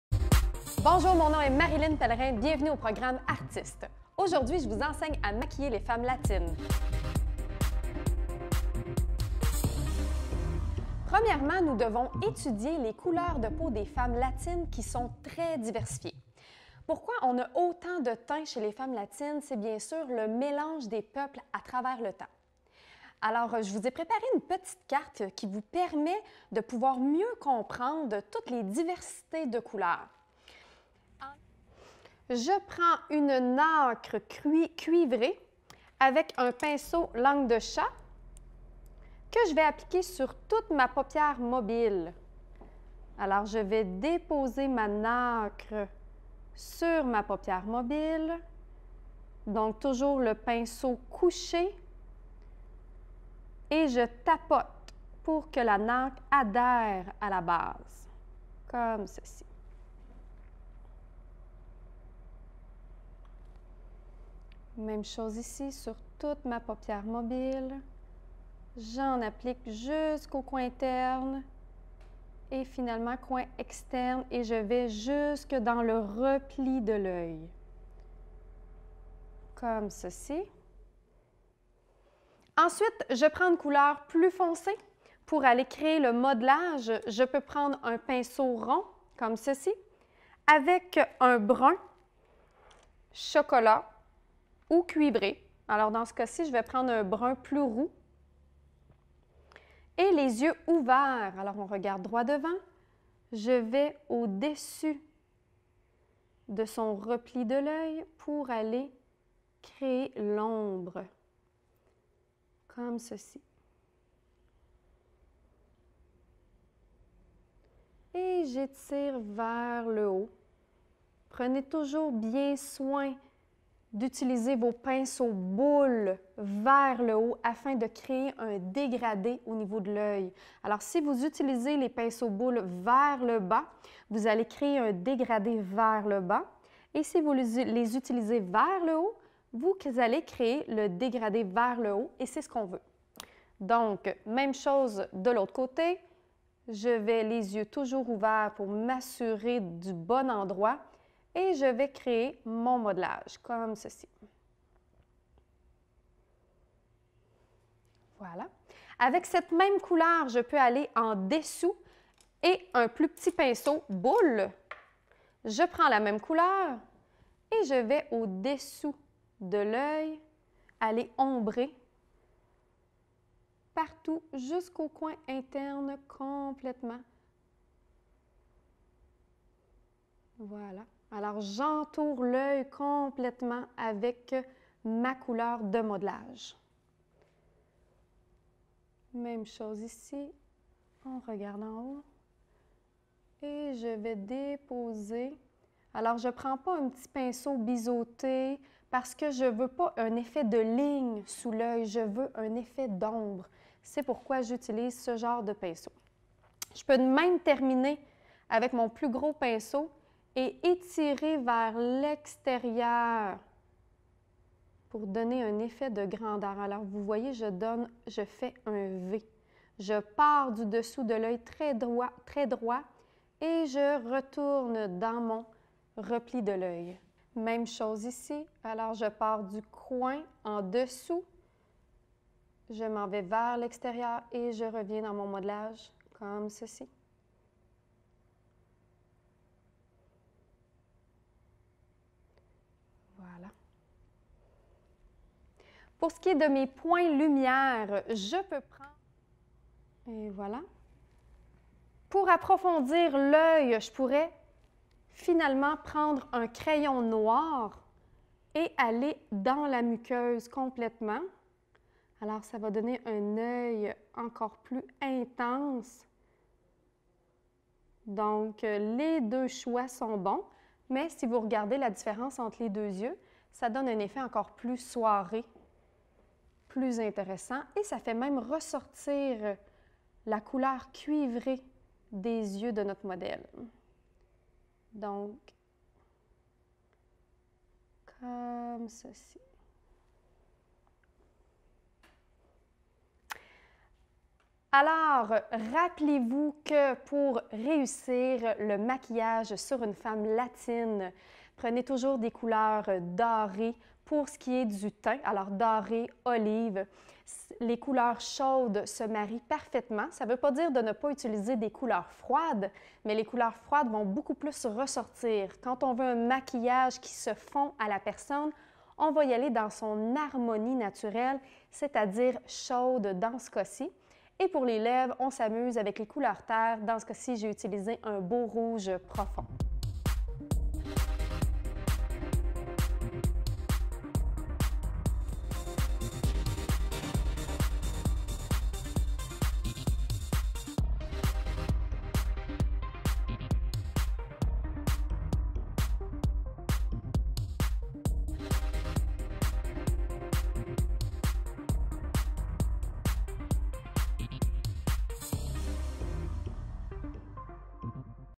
LES FEMMES LATINES | Extrait de cours
Dans ce cours, vous découvrirez la morphologie générale des femmes latines. Nous vous enseignerons leur différentes teintes et sous teintes de peau ainsi que quelques astuces à ne pas oublier. vous apprendrez étape par étape comment obtenir un maquillage adapté à la femme latine.